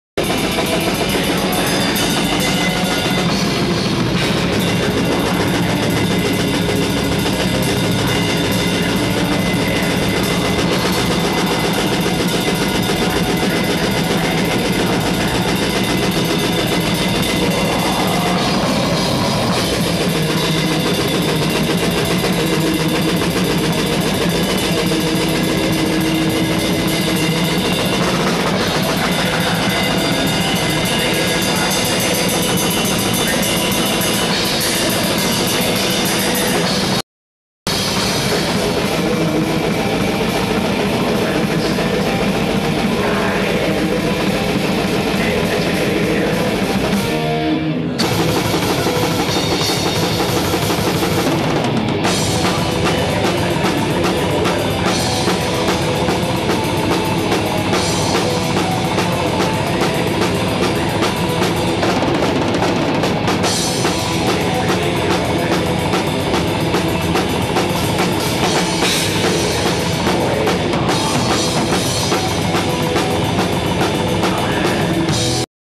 Satyricon - Frost drumming - Drums
Il batterista dei Satyricon in sala prova.
Troppo bravo, troppo un grande!!